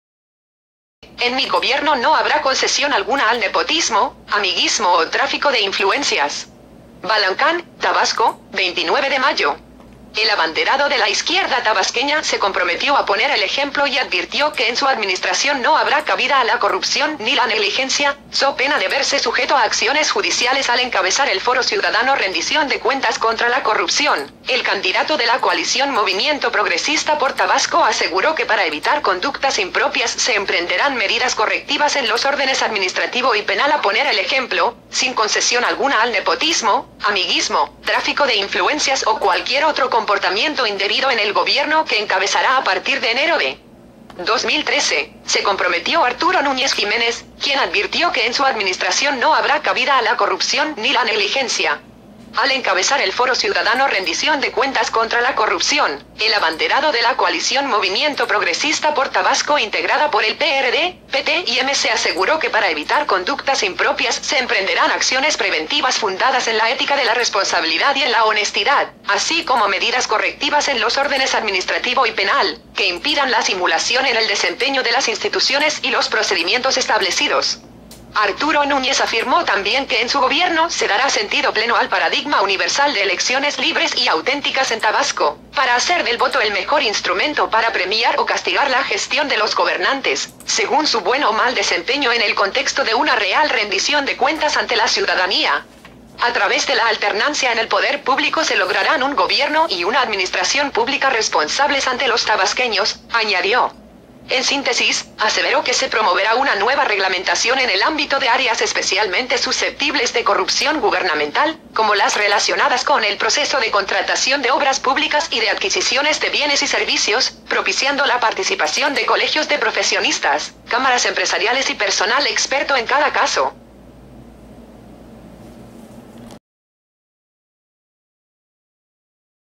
En mi gobierno no habrá concesión alguna al nepotismo, amiguismo o tráfico de influencias.mpg
En mi gobierno no habrá concesión alguna al nepotismo, amiguismo o tráfico de influencias.
Balancán, Tabasco, 29 de mayo.-
El abanderado de la izquierda tabasqueña se comprometió a poner el ejemplo y advirtió que en su administración no habrá cabida a la corrupción ni la negligencia, so pena de verse sujeto a acciones judiciales
Al encabezar el Foro Ciudadano Rendición de cuentas contra la corrupción, el candidato de la coalición Movimiento Progresista por Tabasco aseguró que para evitar conductas impropias se emprenderán medidas correctivas en los órdenes administrativo y penal
A poner el ejemplo, sin concesión alguna al nepotismo, amiguismo, tráfico de influencias o cualquier otro comportamiento indebido en el gobierno que encabezará a partir de enero de 2013, se comprometió Arturo Núñez Jiménez, quien advirtió que en su administración no habrá cabida a la corrupción ni la negligencia.
Al encabezar el Foro Ciudadano Rendición de cuentas contra la corrupción, el abanderado de la coalición Movimiento Progresista por Tabasco --integrada por el PRD, PT y MC-- aseguró que para evitar conductas impropias se emprenderán acciones preventivas fundadas en la ética de la responsabilidad y en la honestidad, así como medidas correctivas en los órdenes administrativo y penal, que impidan la simulación en el desempeño de las instituciones y los procedimientos establecidos.
Arturo Núñez afirmó también que en su gobierno se dará sentido pleno al paradigma universal de elecciones libres y auténticas en Tabasco, "para hacer del voto el mejor instrumento para premiar o castigar la gestión de los gobernantes", según su buen o mal desempeño en el contexto de una real rendición de cuentas ante la ciudadanía. A través de la alternancia en el poder público se lograrán un gobierno y una administración pública responsables ante los tabasqueños, añadió.
En síntesis, aseveró que se promoverá una nueva reglamentación en el ámbito de áreas especialmente susceptibles de corrupción gubernamental, como las relacionadas con el proceso de contratación de obras públicas y de adquisiciones de bienes y servicios, propiciando la participación de colegios de profesionistas, cámaras empresariales y personal experto en cada caso.